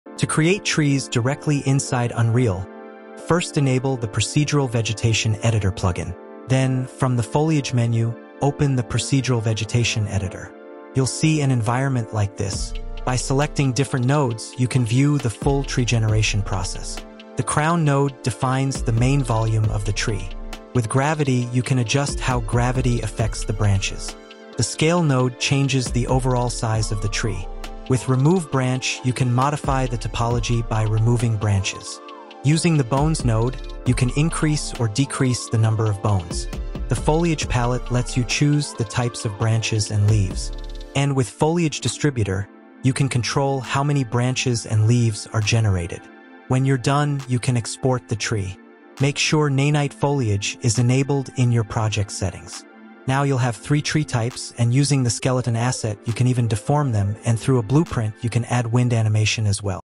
Procedural Vegetation MAGIC in Unreal Engine
In this video, we explore the power of Procedural Vegetation Editor and the new PCG tools in Unreal Engine 5.7 to create a fully dynamic, data-driven forest environment.

Using PCG Graphs, foliage rules, scatter logic, and vegetation layers, we build a forest that updates automatically based on terrain, slopes, masks, and biome rules — giving you total control over your environment.

🌲 What you’ll learn in this episode:
Setting up the Procedural Vegetation Editor
Creating vegetation layers and scatter rules
Using PCG Graphs for adaptive forest generation

This workflow is perfect for environment artists, technical designers, and anyone building large open worlds in Unreal Engine.